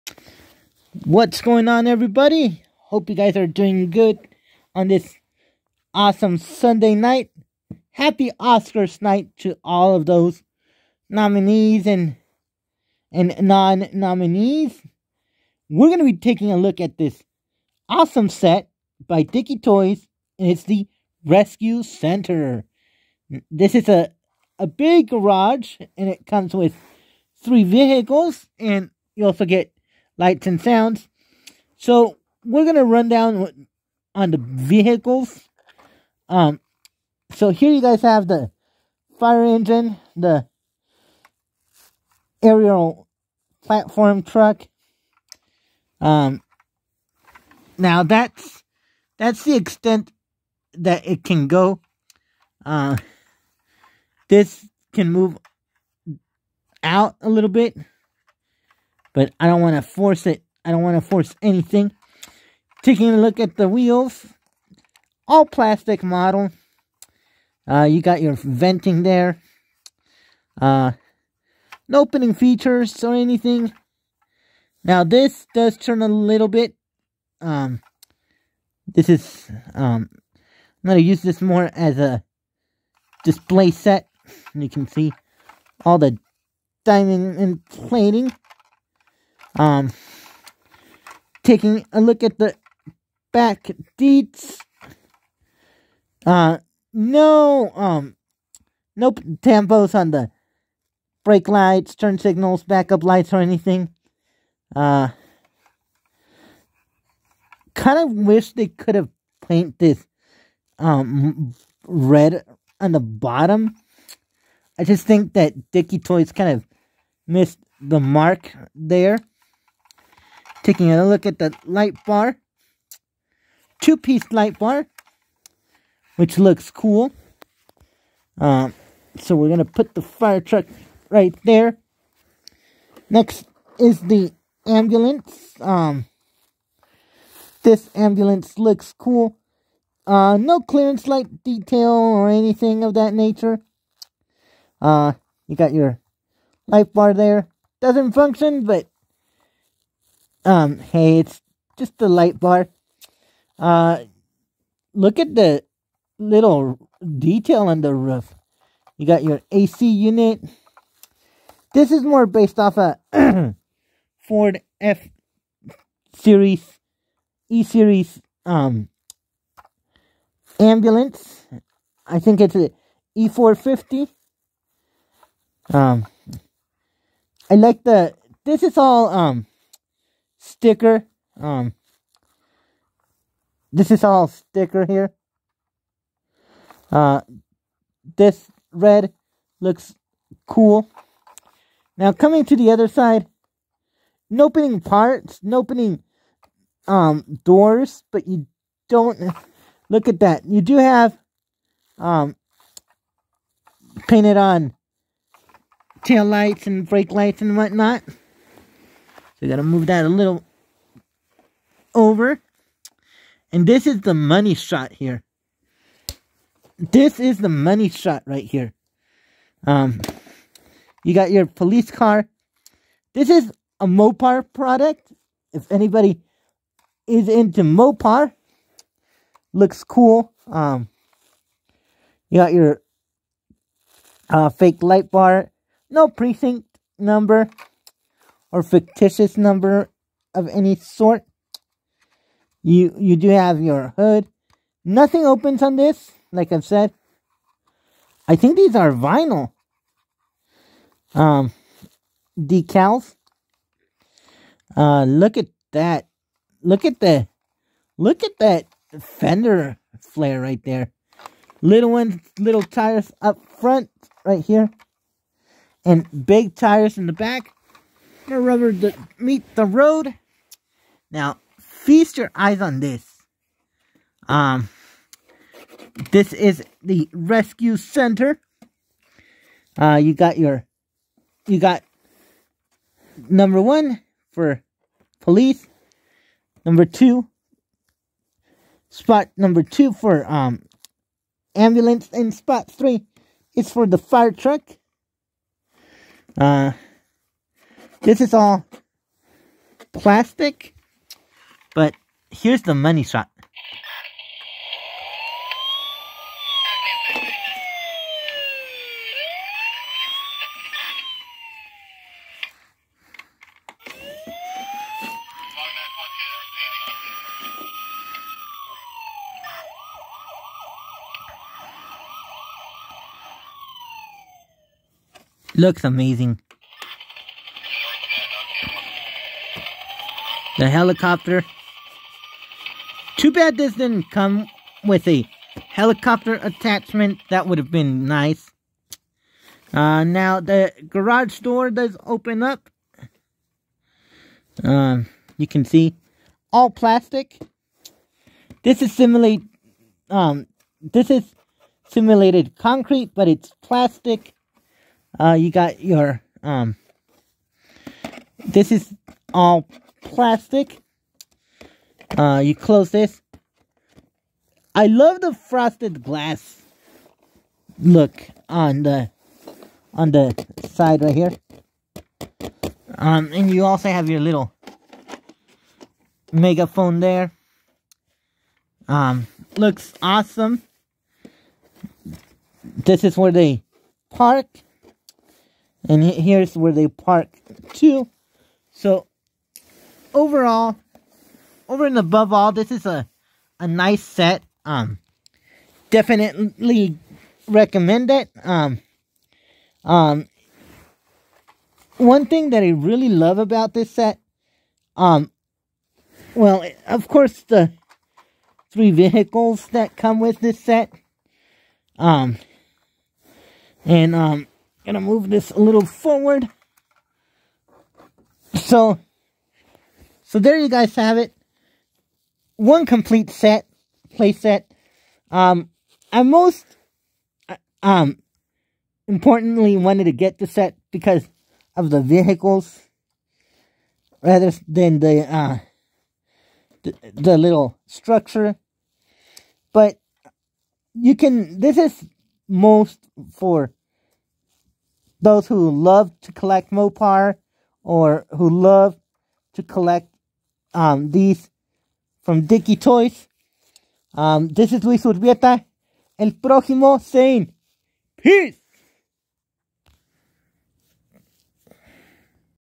Dickie Toys Rescue Center All Plastic Playset with 3 models
Fire truck, Ambulance, and Police Car. Recommend this special set for anyone who loves Emergency Vehicles.